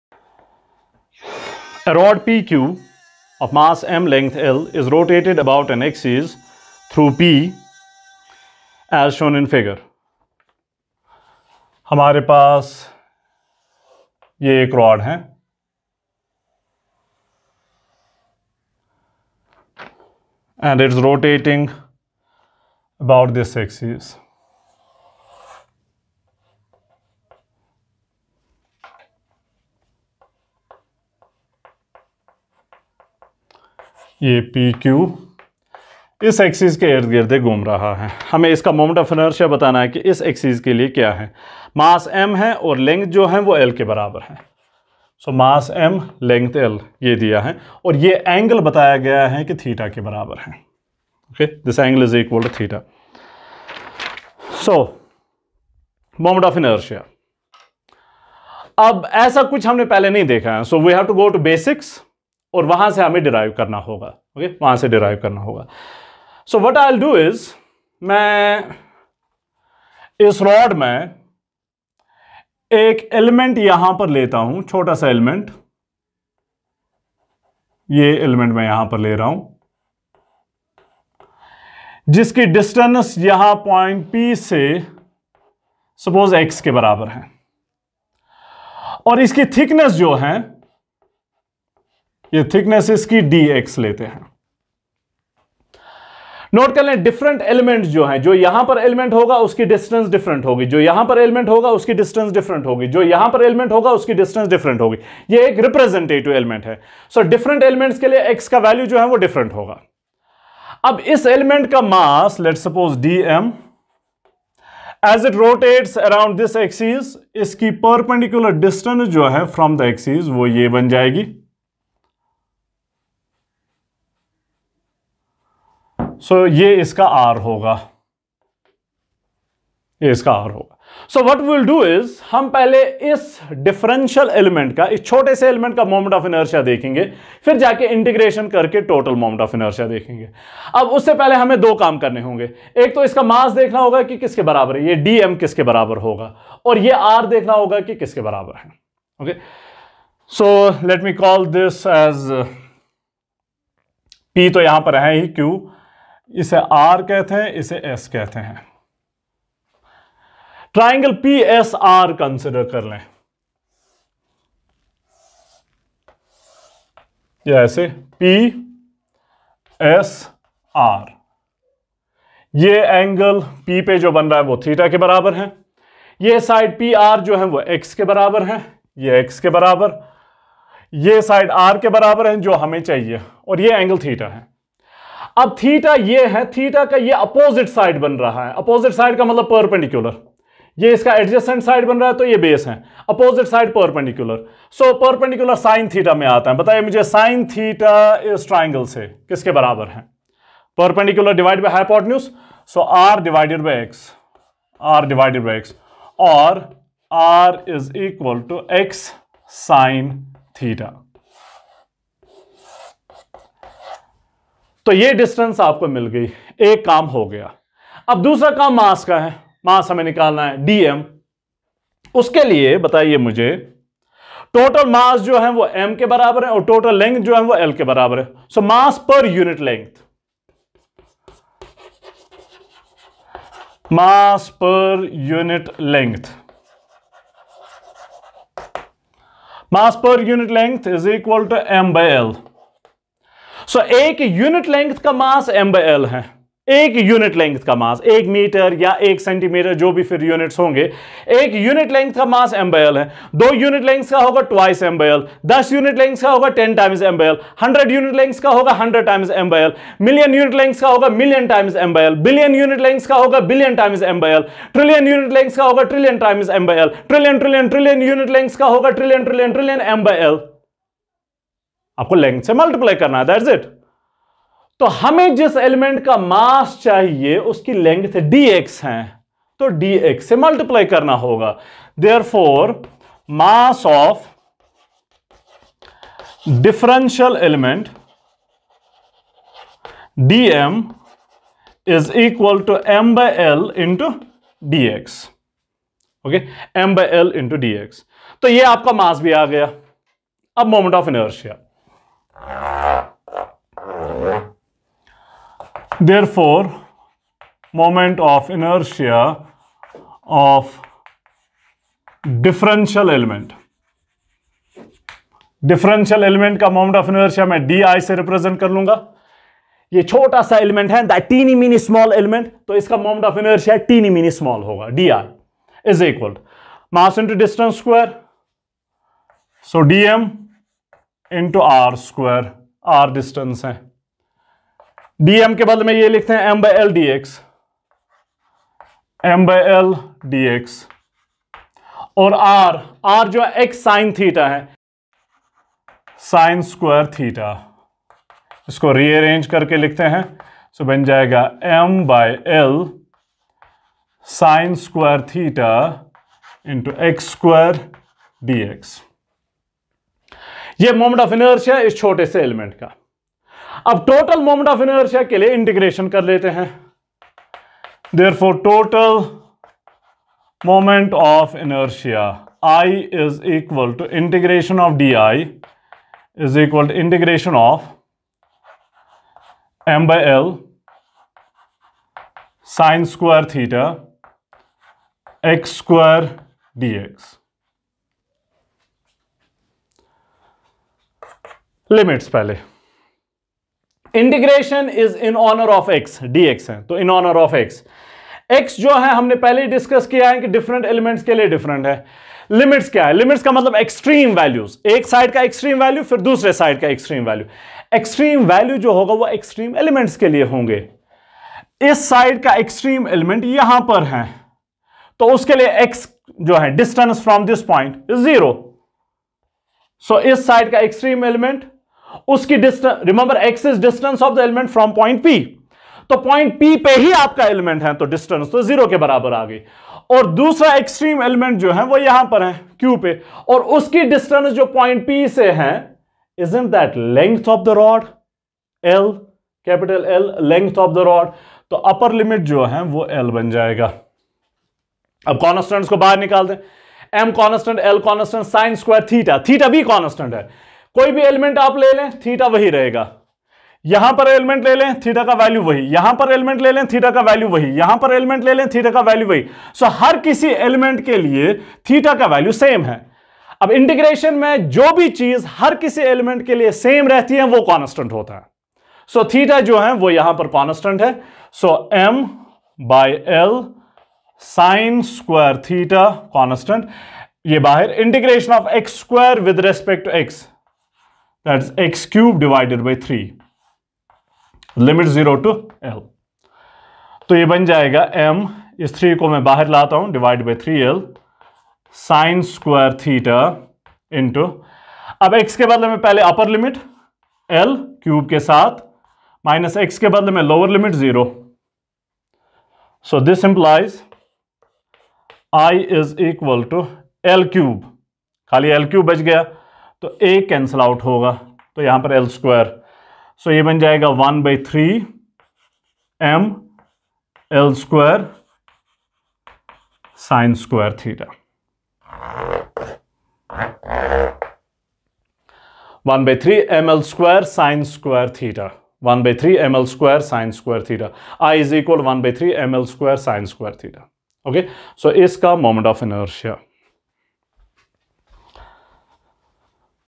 023 EXAMPLE ON M.O.I
A rod PQ of mass ’m ’ and length L IS rotated about an axis through ’P’ as shown in figure. Find the moment of inertia of the rod about the axis of rotation.

grade 11, CBSE/NCERT,  NEET/JEE